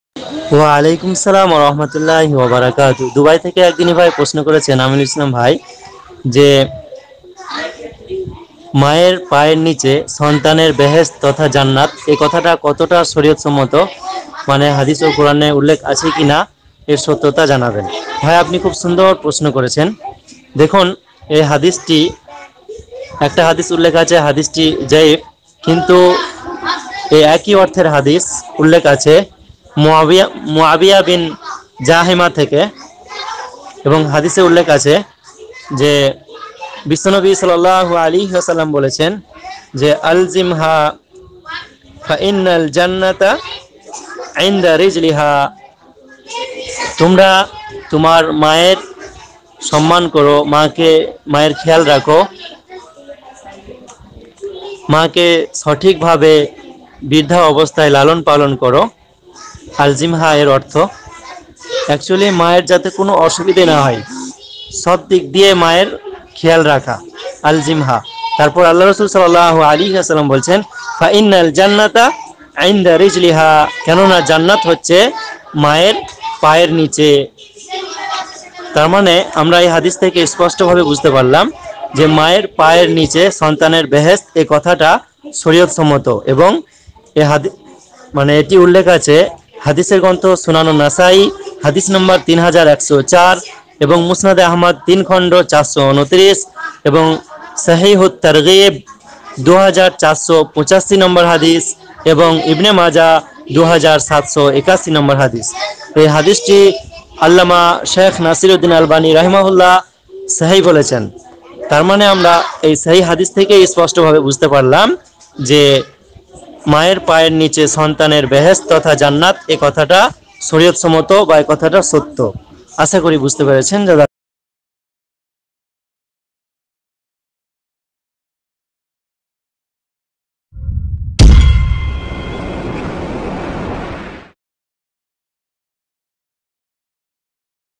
মায়ের পায়ের নিচে জান্নাত এই কথাটি কি শরীয়াত সন্মত ?
প্রশ্নো:- মায়ের পায়ের নিচে জান্নাত এই কথাটি কি শরীয়াত সন্মত ?

Question:- What is this paradise under the feet of mother sarij sanimat ?

উত্তর প্রদান করেছেনঃ শাইখ সাকিব বিন আব্দুল হাকিম আল বুখারী (হাফেঃ)

Answer:- Seikh Shakib bin Abdul Hakim Al-Bukhari  (Hafezahul)

💻 Arrangements: Sunnah Islamic Question @ Answer Group